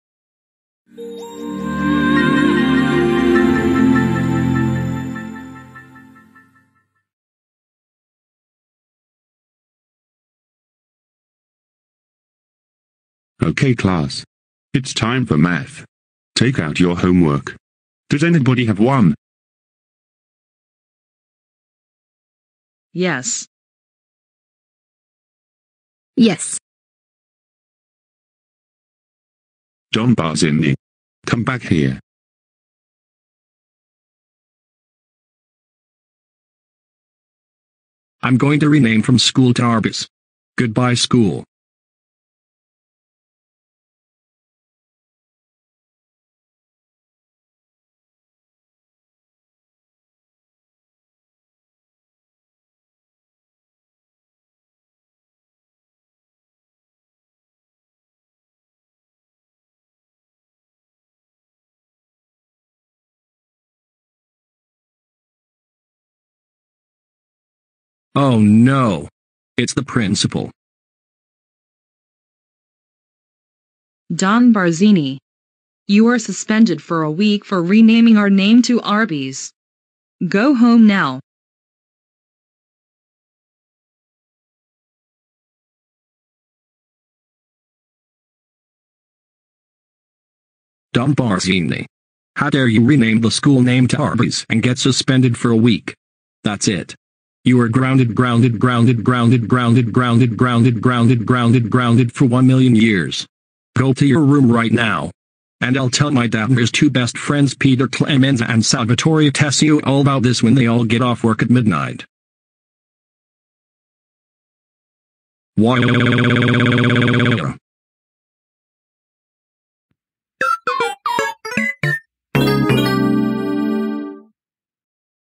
Don Barzini Renames His School Into Arby's And Gets Grounded
Requested by Konstantin Odinokov
Cast:
Steven as Don Barzini and Sonny Corleone
Kate as Yang Xiao Long from RWBY and Cynthia from Pokemon
Simon as Shrek
Veena as Adagio Dazzle from My Little Pony Equestria Girls